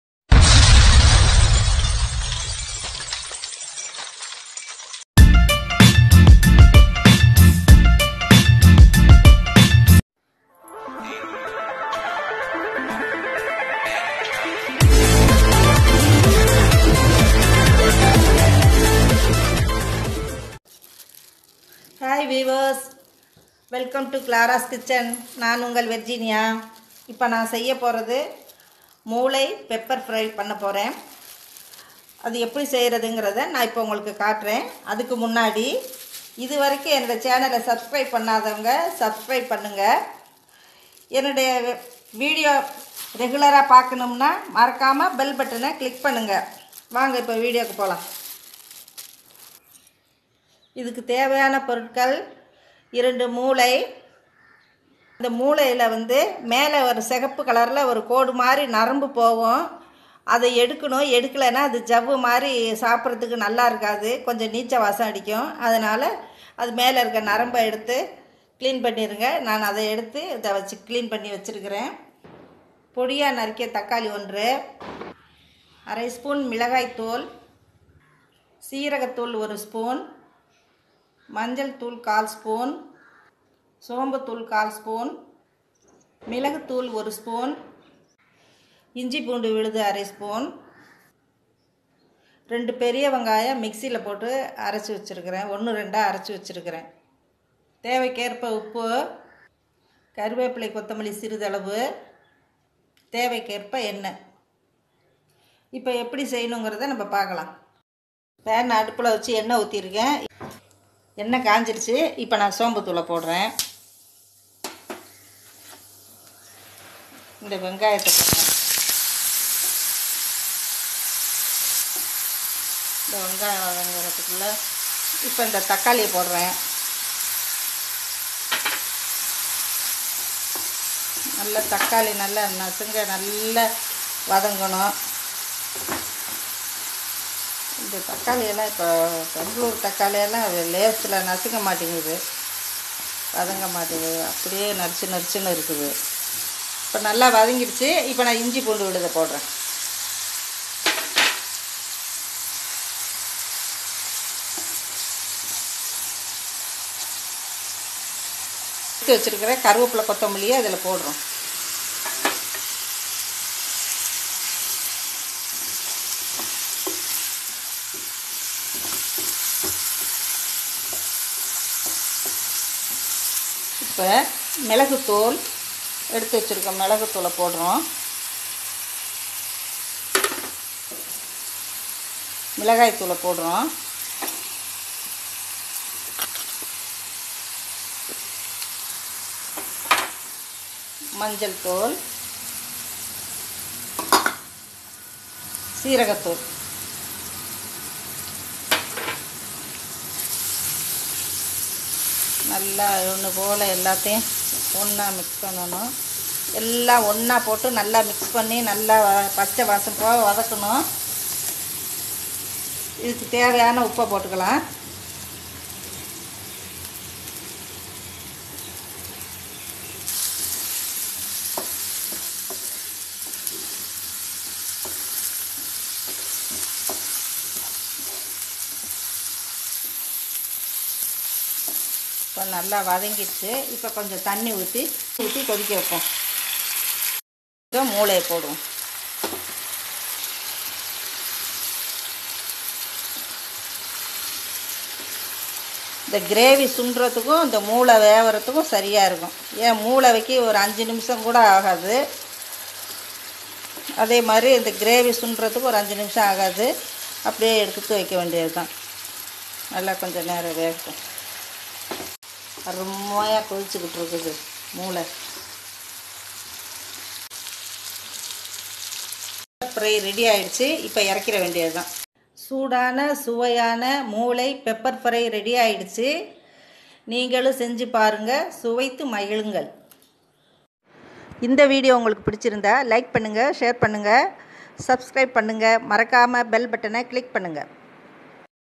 மூளை வறுவல் | Brain Fry in Tamil | goat brain recipe in tamil | Mutton Brain Fry | Brain Pepper fry
Brain fry is a spicy and yummy dish done with pepper. This recipe taste's good. Brain fry recipe is easy to cook and does not consume much of your time. Cooking time does not exceeds 10 minutes.

Following are the ingredients required:
2 nos goat
1 tomato finely chopped
1/2 tl spoon red chilli powder
1 tbl spoon cumin powder
1/4 tbl spoon turmeric powder
1/4 tbl spoon fennel powder
1 tbl spoon pepper powder
1/2 tbl spoon ginger garlic paste
2 onions smashed

This is how it has to be done:

Step 1: Initially rinse the brain and remove the red vein on top of it. Rinse it again.

Step 2: Now heat a pan with oil. Once it is hot add fennel powder/Sombu powder. Add onions to it and saute well. Saute well until it turns golden brown.

Step 3: Now add tomatoes and saute until it turns mushy.

Step 4: Now add ginger garlic paste and saute well. Then add curry and coriander leaves.

Step 5: Now add pepper powder followed by red chilli powder then turmeric powder, then cumin powder. Saute well until raw smell goes off. Add salt as required.

Step 6: Add little water and boil well.

Step 7: Now add the brain to it. Let it boil. Brain does not take much time for cooking. Boiling time must not exceed 5 minutes.

Brain fry is delicate so handle gently. Spicy brain fry recipe is ready to be served. Sit back and enjoy the spicy yummy taste!